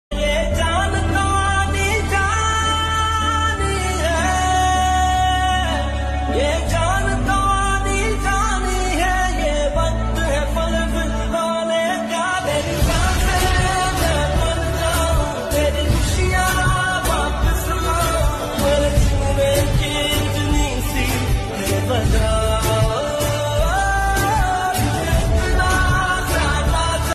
MAJOR MIAN ABDULLAH SHAHEED BURIED WITH MILITARY HONOUR
MAJOR MIAN ABDULLAH SHAHEED FEOM FC KPK WAS MARTYRED TODAY IN IBO .